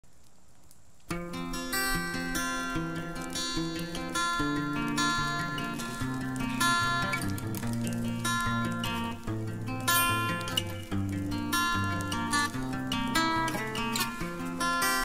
Cleaning up land for tiny shipping container home
Land clearing for tiny shipping container home , on top of river cliffs